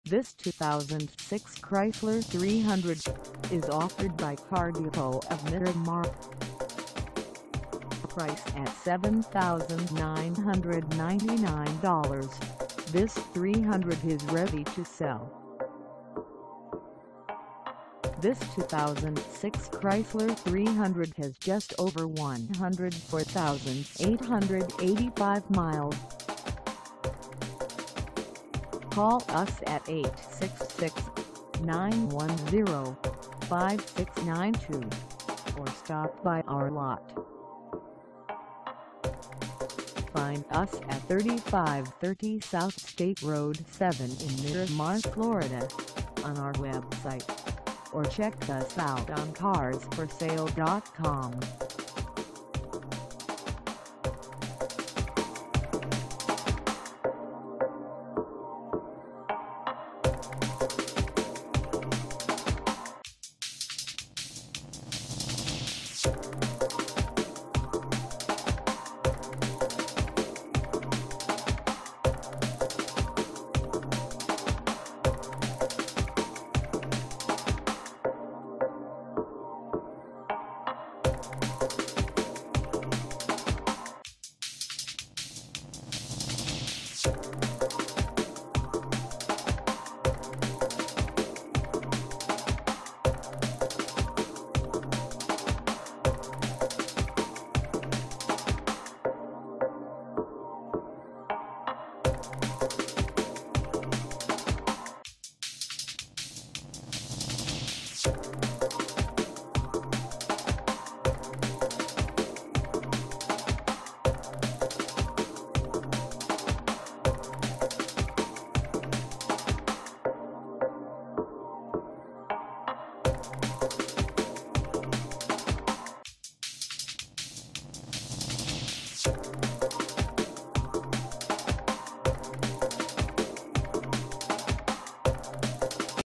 2006 Chrysler 300 for sale in Miramar, FL 33023 at the Car D
This 2006 Chrysler 300 is for sale in Miramar, FL 33023 at Car Depot of Miramar.
Engine: V6 2.7L V6
Transmission: Automatic 4-Speed
Color: Unspecified
Mileage: 104885
Contact Car Depot of Miramar 3530 S STATE ROAD 7 Miramar, FL 33023 to test drive this 2006 Chrysler 300  today.